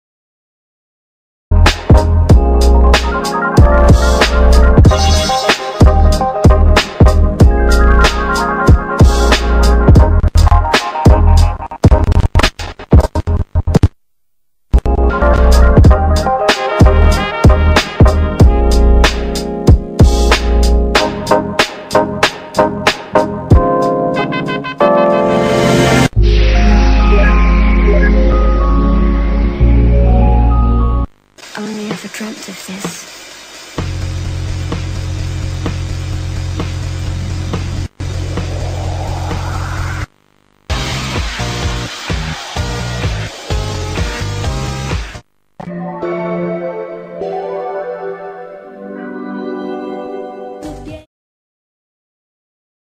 HEX » Inicio de transmisiones 15/05/11 + Gráfica Mayo-Junio 2011
Volvamos atrás...
Hubo un video que salió 4:3 comprimido.
Lo renderizé otra vez, pero incluyo un poco más de la continuidad y del segmento de música 2M, y revisando la PC, encontré el ID que salía en ése entonces, y lo coloqué en éste mismo video...

Actualmente corre otro pack gráfico.

Copyright » Teleimagen18 (Transmision y gráfica on air), TeleArte (Audio del chart "Presenta")